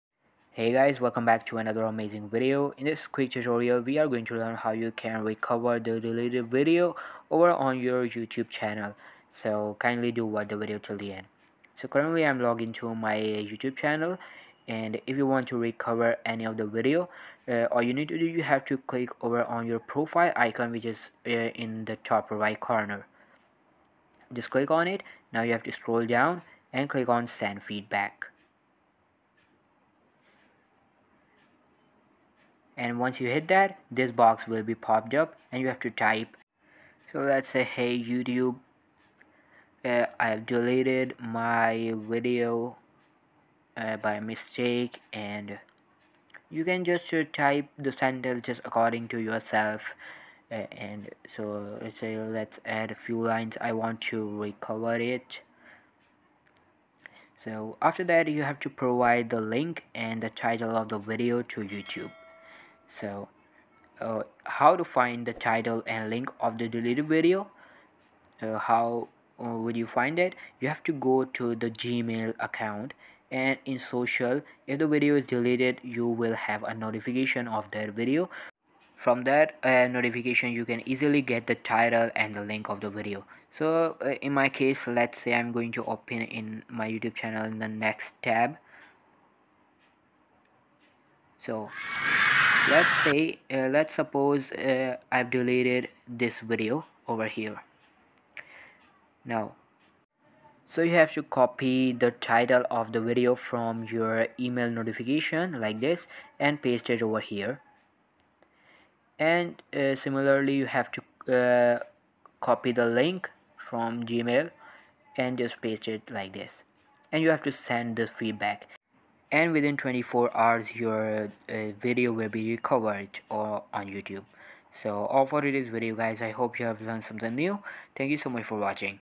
How To Recover Deleted Youtube Videos Quickly in 2023
In this video we tell you how to recover all deleted youtube videos quickly in 2023. If you deleted any videos by a mistake you can get it back. Follow the steps in this guide to get your deleted youtube videos back. This guide is made in 2023.

00:00 Intro
00:10 How to recover deleted youtube videos quickly